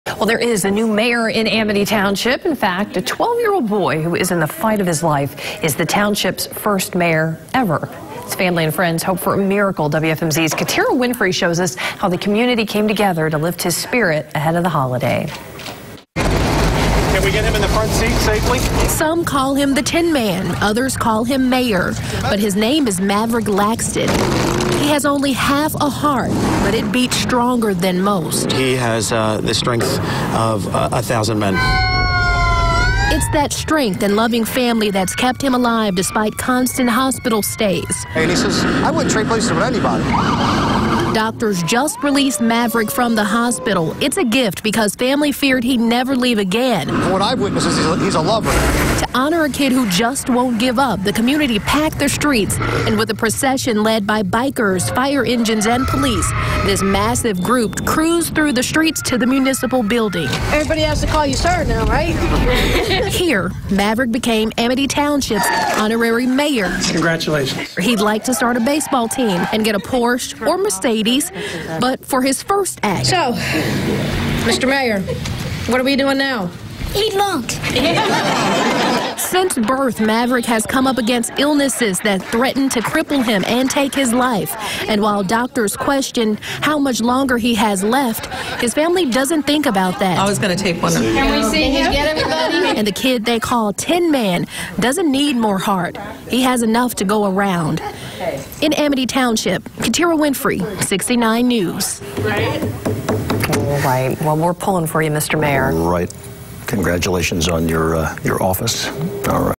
Boy with with half a heart becomes town's mayor
Original airdate: 11/22/2017 | AMITY TWP., Pa. - There's a new mayor in Amity Township. In fact, a 12-year-old boy who's in the fight of his life, is the township's first-ever mayor. As family and friends hope for a miracle, the community came together Wednesday to lift his spirit ahead of the holiday.

Some call him the Tin-man, others call him mayor, but his name is Mavrik Laxton. He has only half a heart, but it beats stronger than most.

"He has the strength of a thousand men, and it's amazing. His drive," said Amity Township police Chief Andrew Kensey.

It's that strength and loving family that's kept him alive, despite constant hospital stays.

C.J. Santangelo, a family friend and organizer with the nonprofit A-Team, recalled a conversation with Laxton while visiting him in the hospital.

"Talking to him, he said, 'I'm OK if I don't live long,' and I said, 'You don't have to think that way,' and he said, 'I wouldn't trade places with anybody.'"